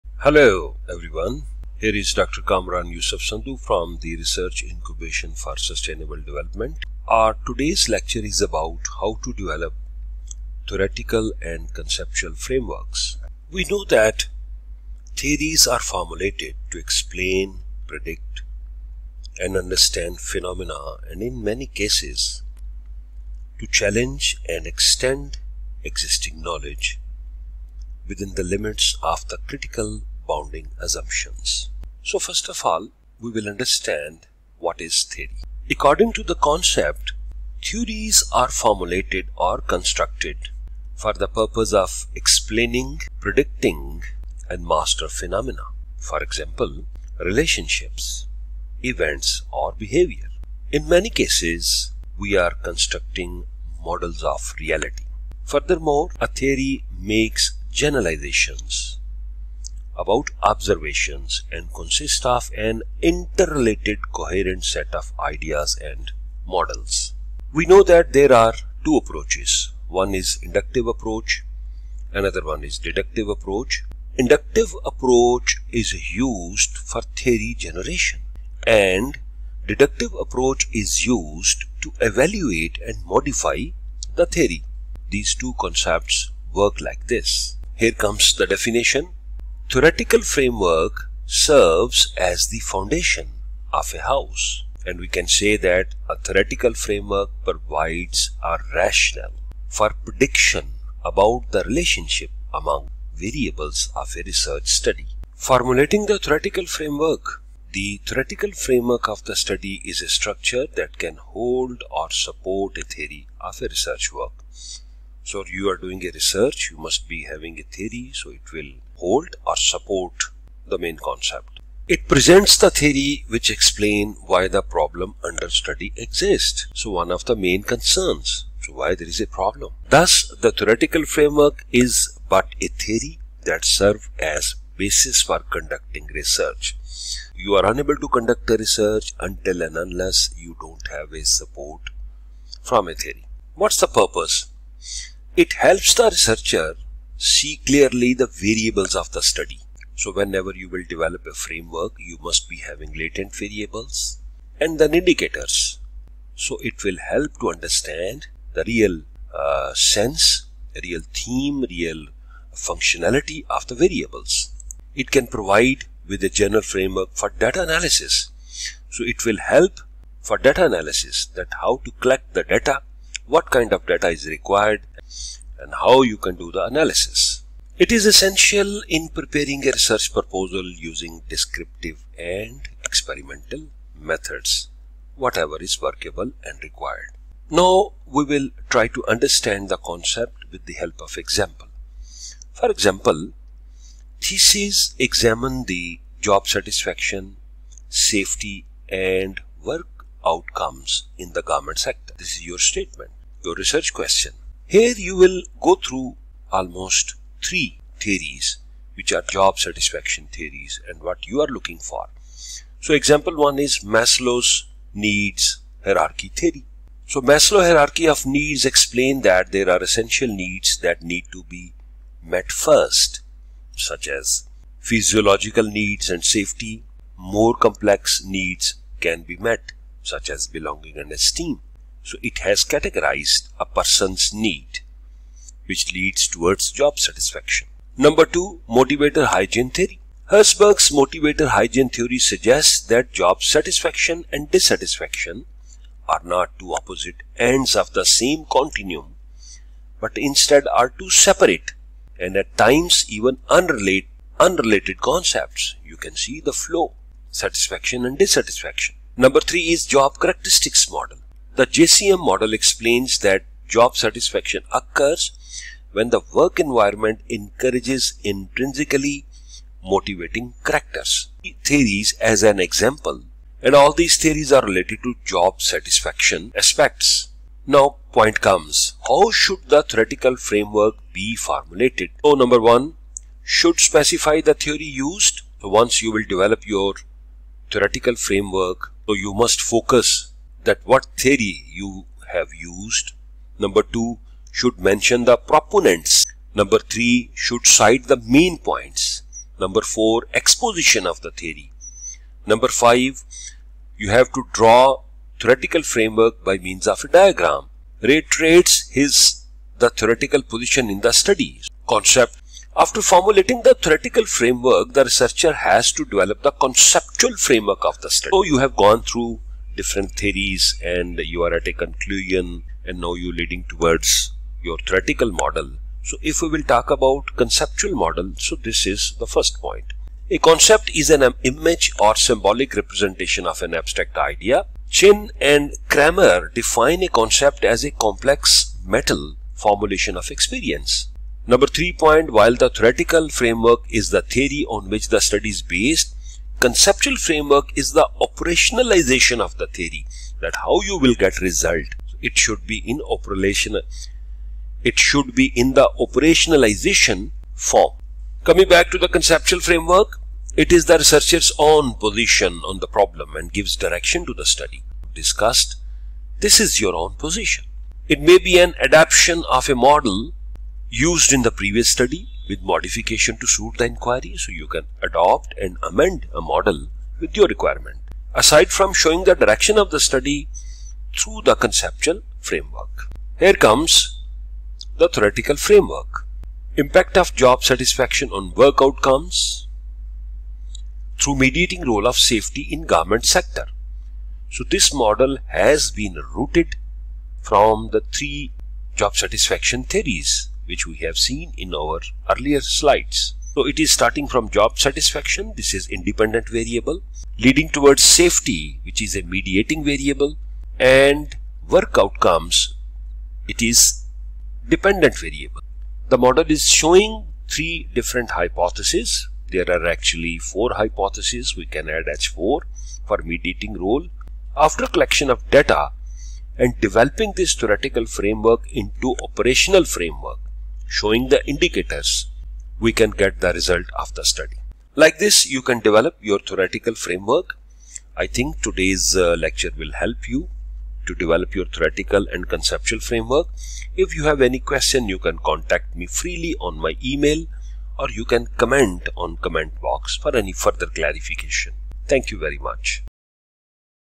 How to Develop Theoretical and Conceptual Framework | Research Method |Dr. Sandhu | The RISD
Key steps to develop Theoretical and Conceptual Framework | Dr. Sandhu | The RISD

Recommended Lectures

Right approach to select the best topic for research | Step by step approach by Dr. Sandhu